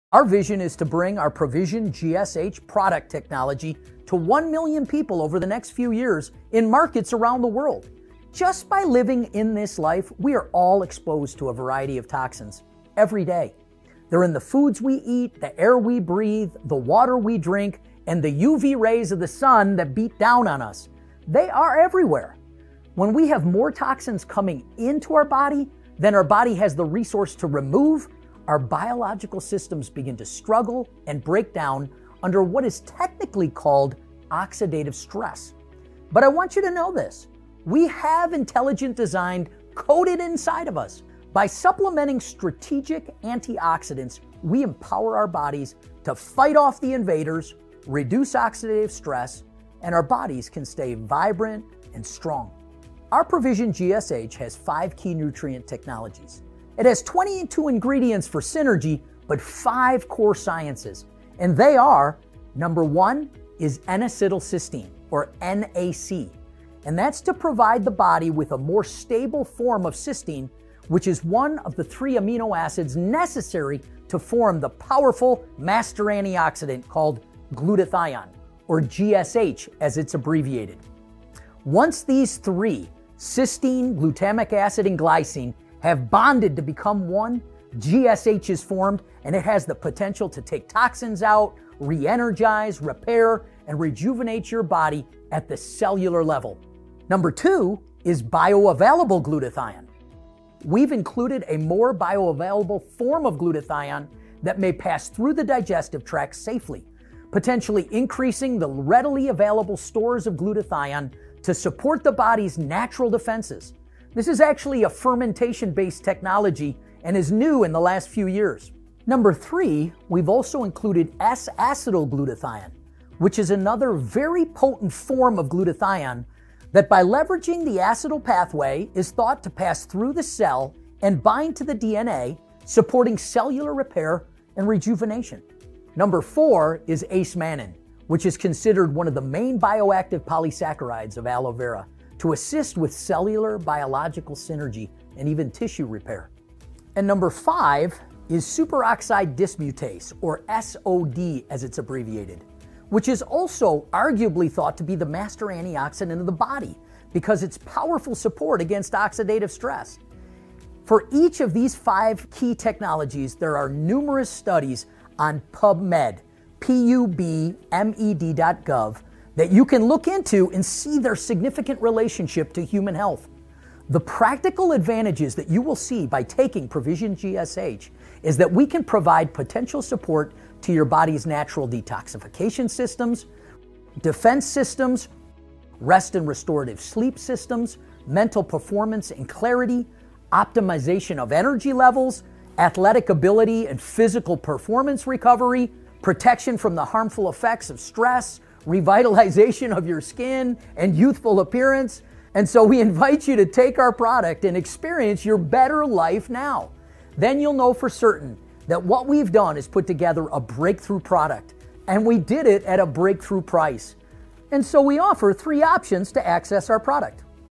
Provizion GSH Health 3.38 min
Everyone suffers from Oxidative Stress.
Provision GSH+ A Powerful Anti-Oxidant giving you energy and slows down the aging process.